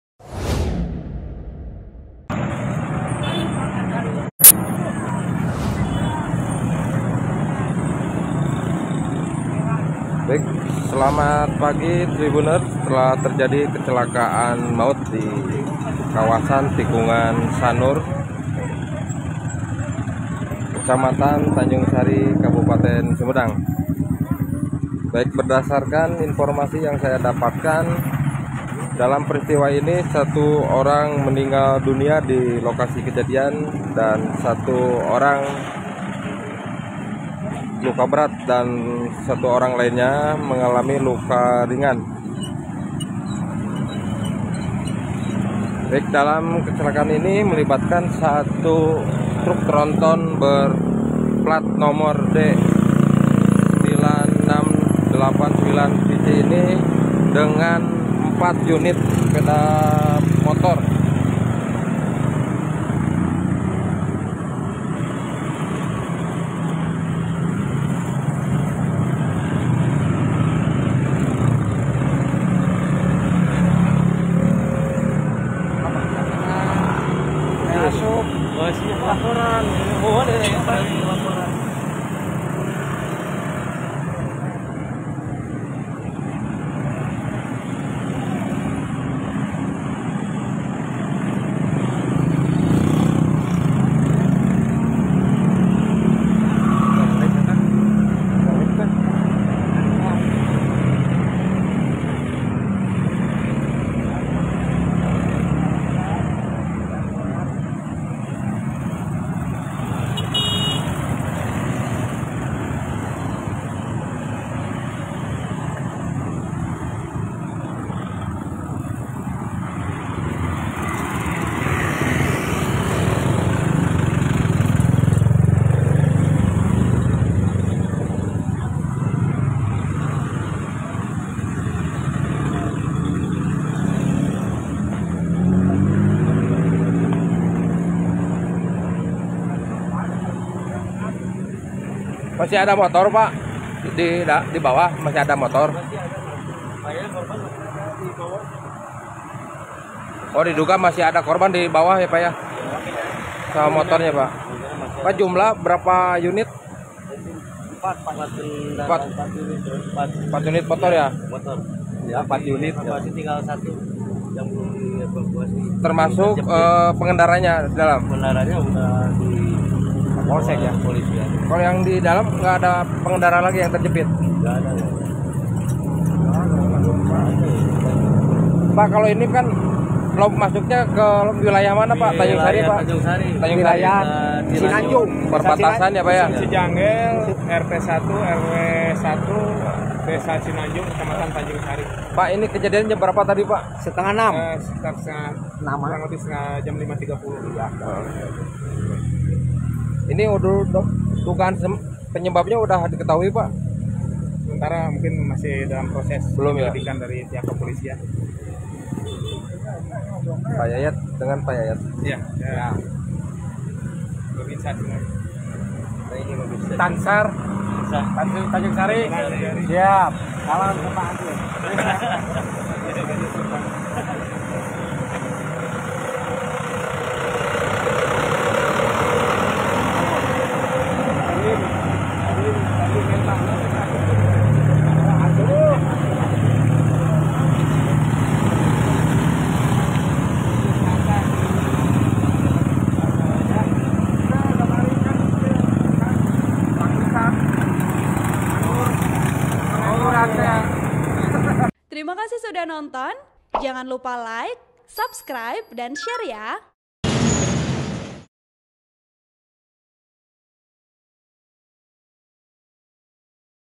KECELAKAAN MAUT DI JATINANGOR, TRUK TERGULING 1 ORANG TEWAS
LIVE : BREAKINGNEWS : TRONTON TERGULING DI TIKUNGAN SANUR TANJUNGSARI, 1 TEWAS

Penulis: Kiki Andriana | Editor: Januar Pribadi Hamel | Video Editor: Edwin Tk